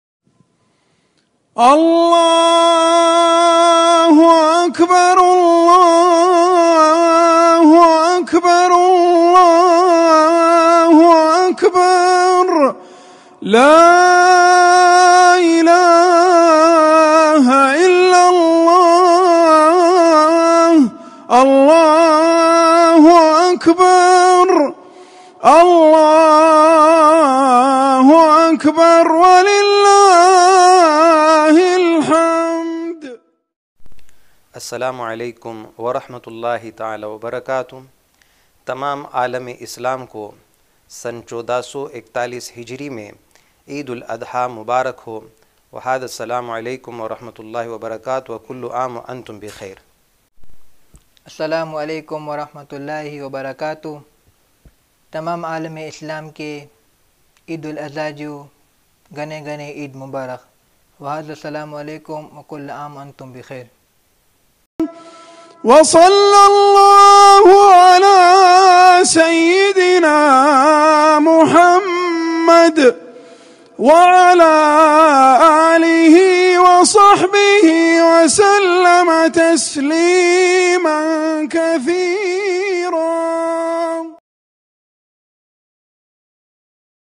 Eid Mubarak To All Muslims Around The World | Bushra Lanaa.
On behalf of Bushra Lanaa Team, we take this beautiful opportunity Wishing you and your family peace, harmony, happiness, good health and prosperity on the auspicious occasion of Eid. Cherish every moment of this beautiful festival and may all your dreams come true soon. Eid Mubarak to youu and everyone at home.💕